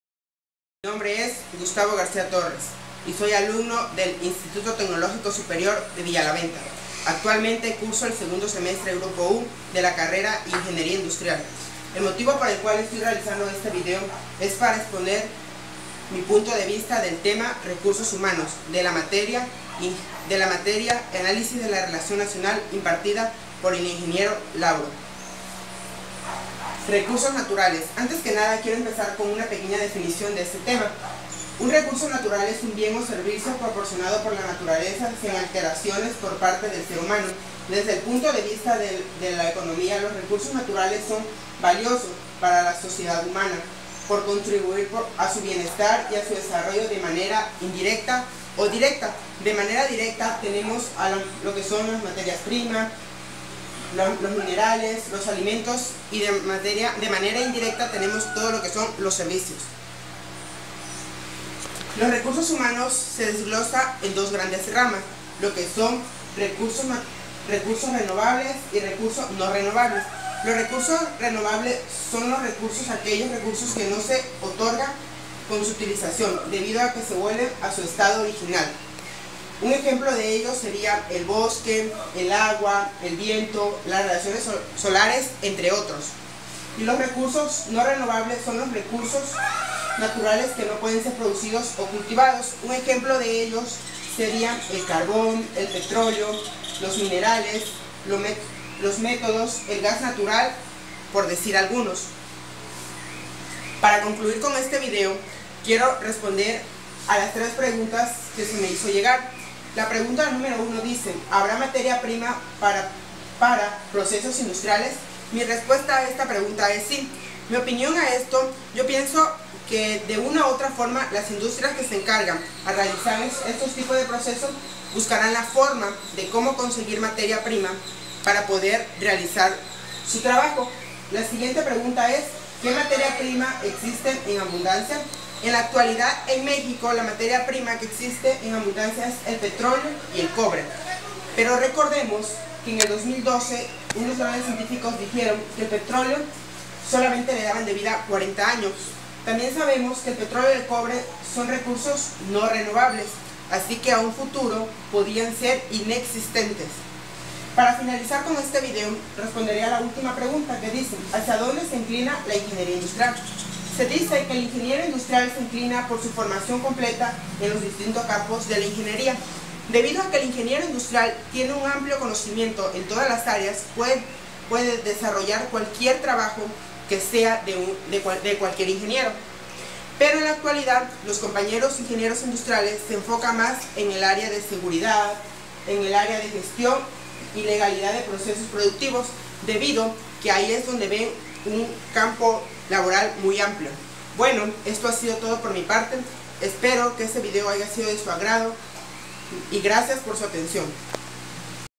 ANÁLISIS DE LA REALIDAD NACIONAL: Recursos Naturales (G. G. T.)
Un recurso natural es un bien, una sustancia o un objeto presente en la naturaleza, y explotado para satisfacer las necesidades y deseos de una sociedad humana. Por lo tanto se trata de una materia prima, mineral (ej.: el agua) o de origen vivo (ej.: el pescado). Puede ser de materia orgánica como el petróleo, el carbón, el gas natural o la turba. También puede tratarse de una fuente de energía: energía solar, energía eólica o, por extensión, de un servicio del ecosistema (la producción de oxígeno vía la fotosíntesis, por ejemplo).

Pesca de arenque
Arenque del Atlántico.
(Foto: NOAA)
Un recurso natural puede existir como una entidad separada como es el caso del agua dulce y el aire así como un organismo viviente como un pez, o puede existir en una forma alternativa que debe procesarse para obtener el recurso como en el caso de los minerales metálicos, el petróleo y la mayoría de las formas de energía.

Desde la década de 1970, esta noción de recursos natural ha evolucionado y tiende a expandirse hacia los recursos útiles para cualquier ecosistema y para todos los sectores socio-económicos. De manera que las superficies disponibles de suelo, la calidad del agua o del aire, el aspecto de los paisajes, la biodiversidad... son otros aspectos de los recursos naturales.